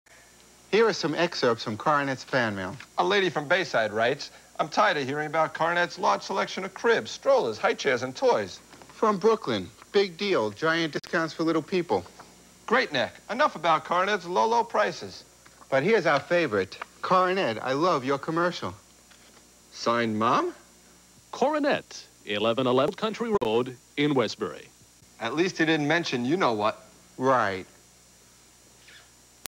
80s Commercial | Coronet | Long Island store | No Talking Orangutans | 1984
1984 commercial for Coronet children's furniture in Westbury, Long Island. remember "No Talking Orangutans!"